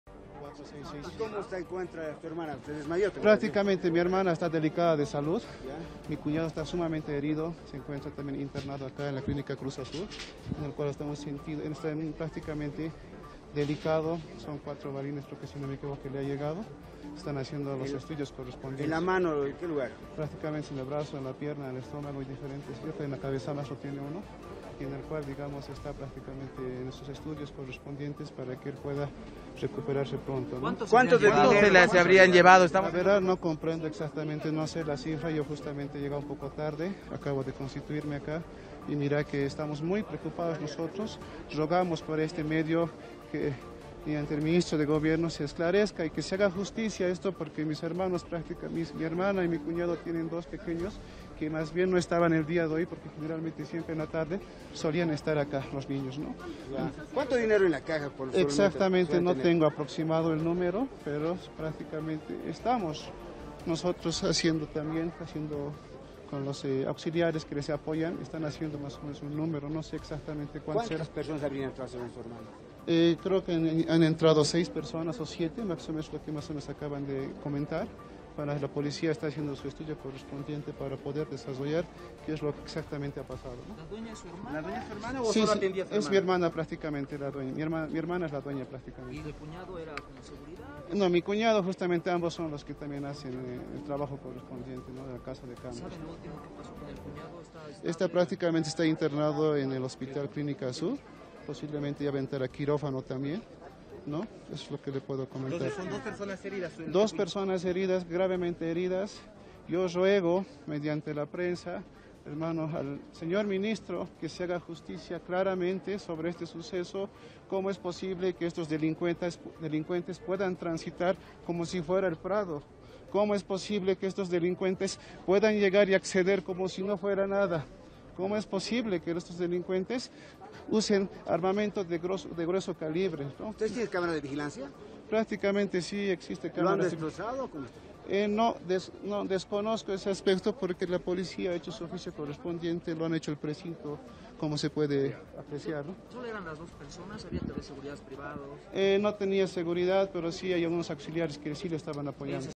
Aún se cuantifica el monto sustraído por los delincuentes a la casa de cambios en El Alto
ElAlto | Pareja de esposos, propietarios de la casa de cambio atracada, tienen varios impactos de bala. Aún se cuantifica el monto sustraído por los delincuentes.